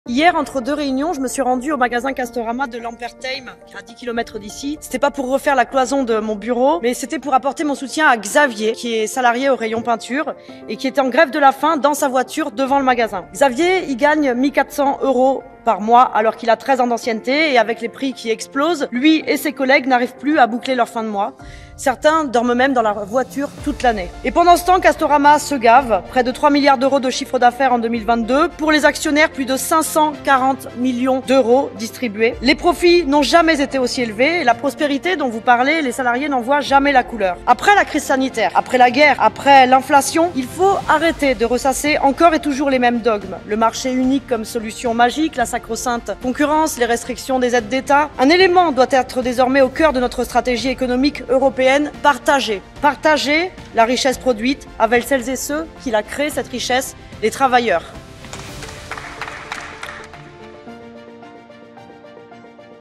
Ce salarié arrête de manger pour protester !
Xavier, salarié au rayon peinture de Castorama depuis 13 ans est en grève de la faim pour dénoncer ses conditions de travail.

Castorama, c'est 540 millions de dividendes en 2022 alors que certains de leurs salariés dorment dans leur voiture, faute d'un salaire décent.

Quand est-ce qu'on partage ?